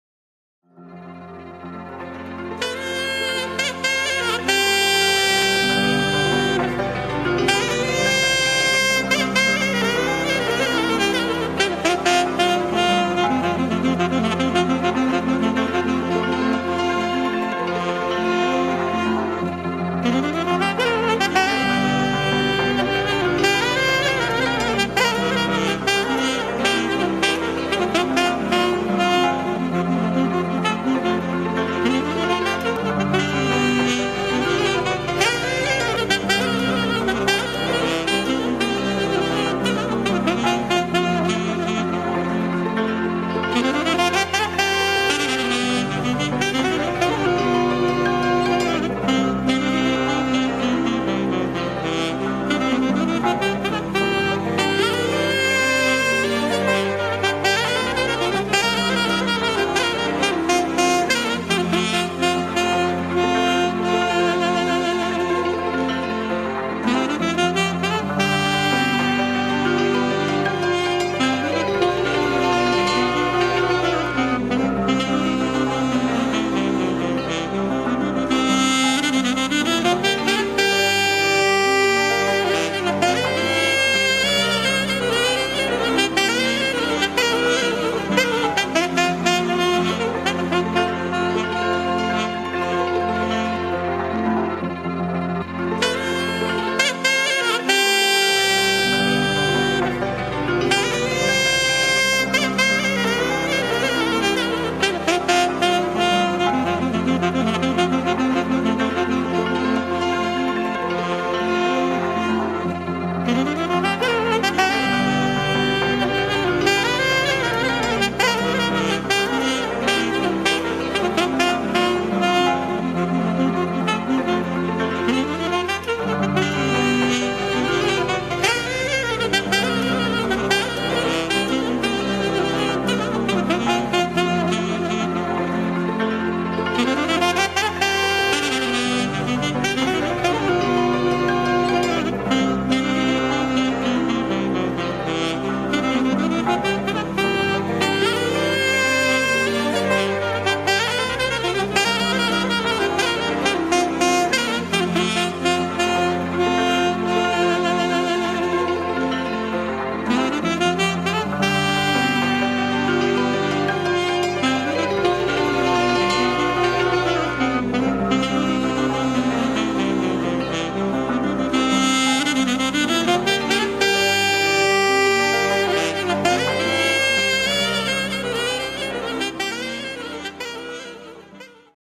Negreanu Marian Doina la saxofon MH.
Acompaniament ,sonorizare si orchestratie Catalin Necsoiu din Severin.